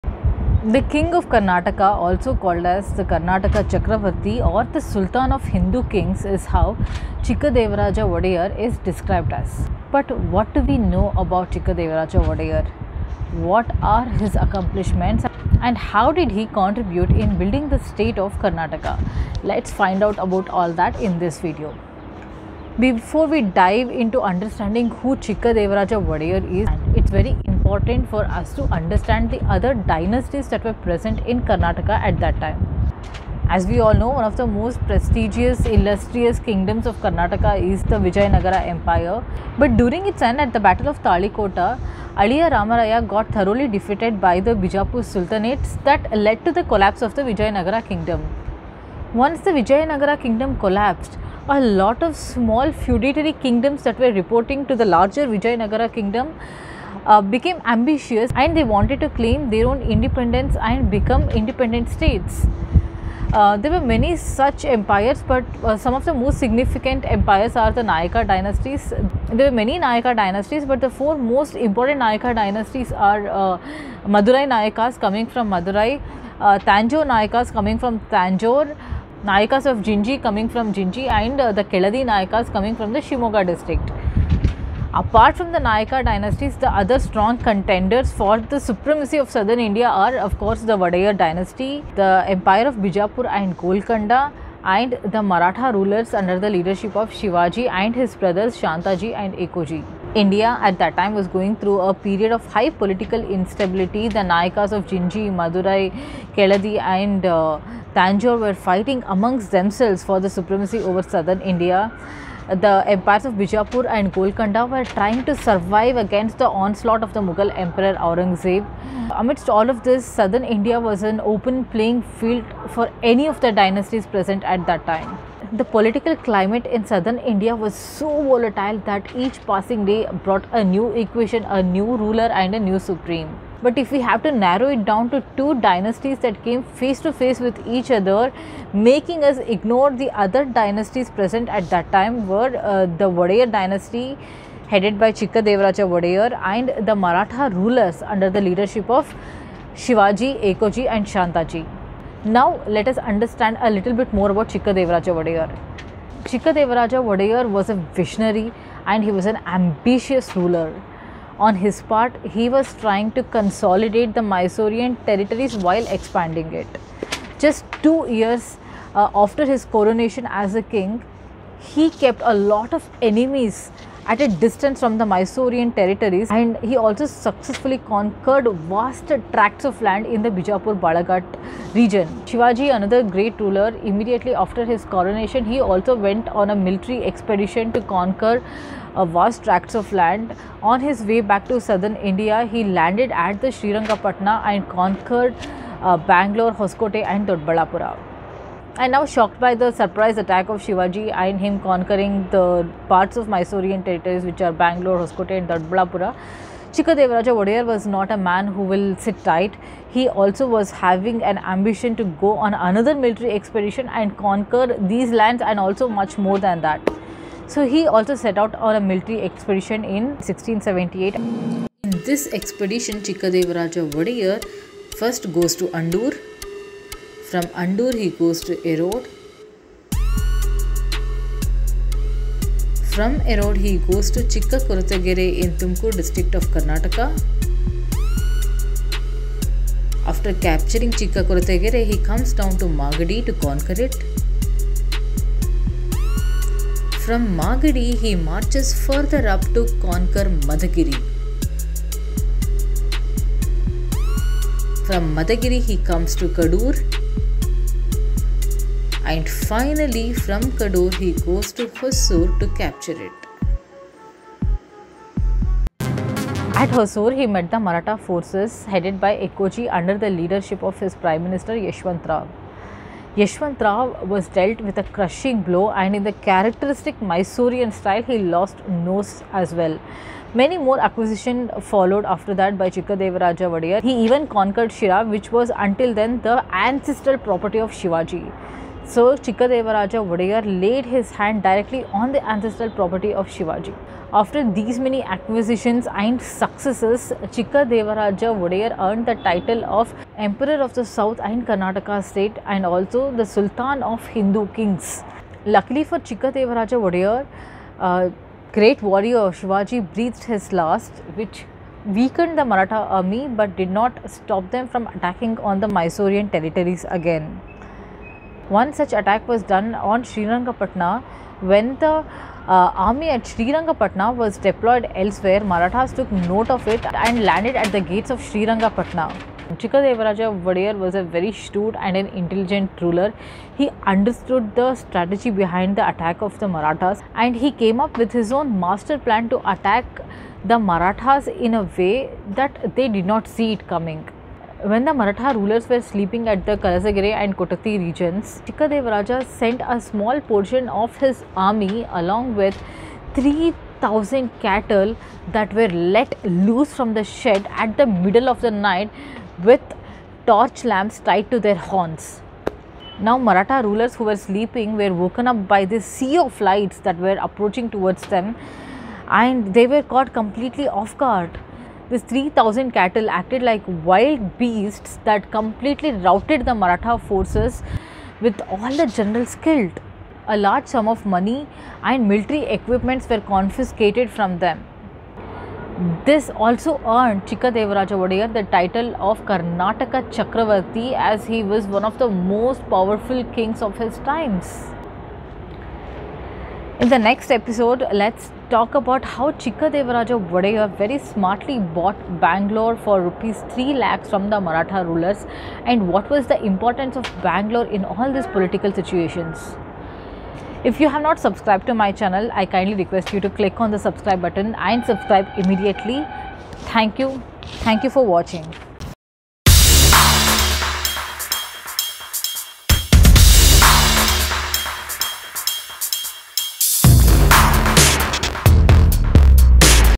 Untold story of Chikkadevaraja Wodeyar|Mysore History in English|Wodeyars of Mysore|Indian History
As we all know one of the most significant dynasties of Karnataka that was prior to the Wodeyars dynasty is the illustrious Vijayanagara dynasty. But during its end, at the battle of Talikota, Aliya Ramaraya from the Vijayanagara empire was thoroughly defeated by the Deccan Sultanates that led to the collapse of Vijayanagar empire. This created a huge vacuum for other rulers to become independent and claim their supremacy.  One of the most important dynasties that emerged from this vacuum is the Nayaka dynasties. The nayakas were originally military governors under the Vijaynagara empire. But when it collapsed, they sprung up and became important contenders to be the next big dynasty. The nayak dynasty was divided into many divisions, but some of the main nayaka dynasties are the Madurai Nayakas, Nayakas of Gingee, Nayakas of Tanjore, Nayakas of Keladi from the shimogga district.
Apart from the nayakas, the other dynasties that were eyeing for the supremacy over southern India were the Wodeyars of Mysore, The empires of Bijapur and Golconda, and the Marathas under the rule of Shivaji and his brothers Ekoji and Shantaji.
India at that time was going through a period of high political instability. The nayakas were fighting amongst themselves, The empires of Bijapur and Golconda were struggling for their survival against the onslaught of the Mughal emperor Aurangazeb. Amidst all of this, the Deccan or South India was an open playing-field for any of the dynasties present at that time.
The political climate in the Deccan was so volatile that each passing day brought new equations, new rulers and new Supremes. But the two major forces that came face to face with each other, making us ignore other dynasties,  and were vying for the same throne of supremacy were the Wodeyars of Mysore, headed by Chikka Devaraja Wodeyar and the multiple rulers of the Marathas.  Mainly Shivaji and his brothers Ekoji and Shantaji. There was a direct contest between the two to win over the Southern India.
Chikka Devaraja Wodeyar was a visionary, and an ambitious ruler. On his part, he was consolidating and also expanding the boundaries of mysore. Just after two years of his coronation as a King, he managed to keep many rulers at a distance from the Mysorean territories and conquered vast tracts of land in the Carnatic – Bijapur Balaghat region.
Shivaji after his coronation as a King, he went on a military tour to expand his territory. He swept the Carnatic in 1677. On his way to the south, Shivaji reached the gates of Srirangapatna, and defeated the wodeyars and conquered the territories of Bangalore, Doddaballapur and Hosakote.
Shocked by the violent surprise attack of Shivaji, Chikkadevaraja wanted to swing into action. A series of attacks and conquests followed. To assert himself as the ruler supreme, Chikkadevaraja set out on an expedition: in 1678, he captured Andur, erode, chikkatotlagere, korategere, Magadi, Maddagiri, Kudur and Hosur from the Nayaks of Madurai.
At Hosur, he met the maratha forces headed by Ekoji uder his prime minister, Yeshwant Rao. Yeshwant was dealt with a crushing blow and in characteristic mysorean style lost his nose as well. Many more conquests continued thereafter, including acquiring Sira, which was until then the ancenstral property of Shivaji.
After these many acquisitons and successes, Chikkadevaraja, earned the title as “the emperor of South and the Karnataka Country.” Or the “Sultan of Hindu Kings.”
Luckily, for Chikkadevaraja, the valiant warrior Shivaji also breathed his last. However, that did not stop the marathas from attacking the Mysorean territories to restore their honour. One such attack by the marathas was done on sriranangapatna at 1682. When the army at srirangapatna was deployed to attack a territory elsewhere, the maratha rulers took note of the situations and tried to attack srirangapatna.
Chikkadevaraja being a shrewd ruler understood the strategy behind the attack of the marathas, made a master counter attack them in a way that they had not expected.
When the maratha rulers were sleeping in Kalasagere and kotthathi regions, chikkadevaraja sent a small portion of his army along with 3 thousand of cattle that were let loose from the shed with torch lamps tied to their horns in the middle of the night.
The maratha generals were woken by this sea of lights that were approaching towards them and they were caught off-guard by this sudden attack. The generals were killed, the maratha forces were completely routed and a huge sum of money, military equipment was confiscated from them. This made Chikkadevaraja Wodeyar as one of the most powerful kings of his times also earning him the title “Karnataka Chakravarthi”